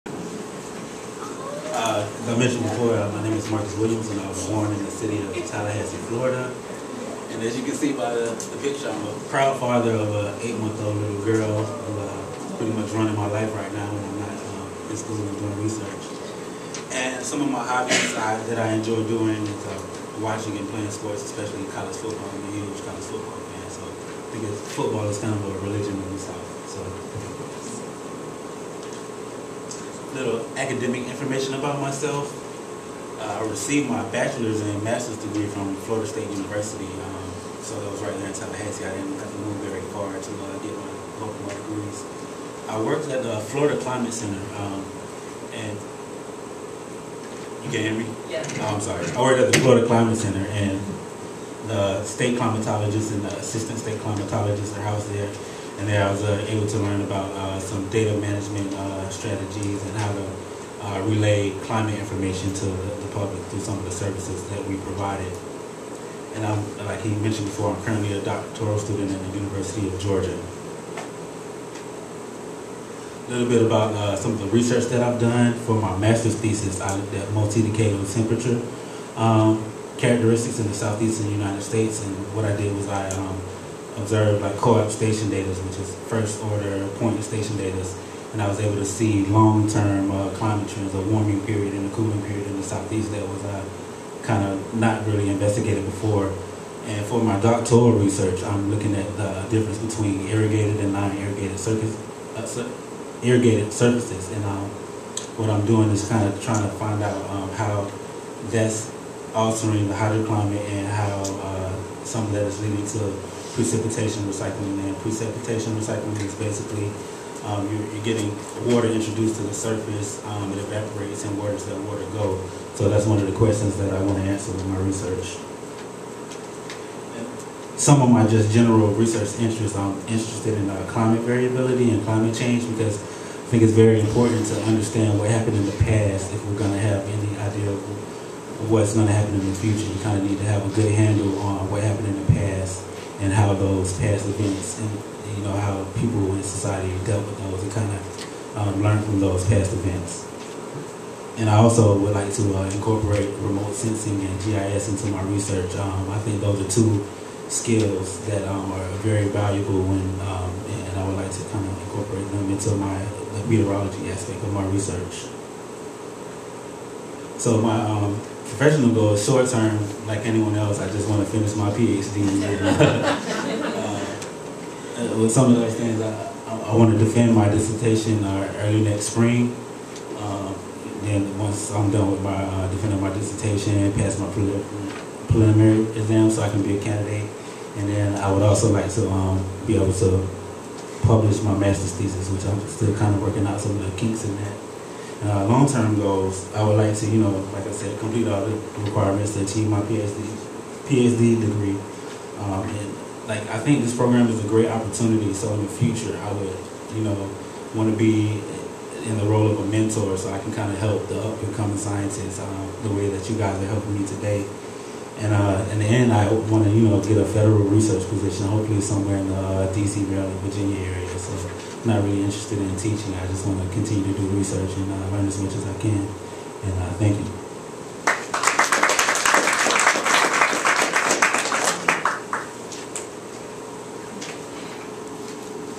MS PHD'S Phase I Presentation Video_Marcus Williams
Marcus Williams, Cohort VIII, Phase I, Five Minute Self-Introduction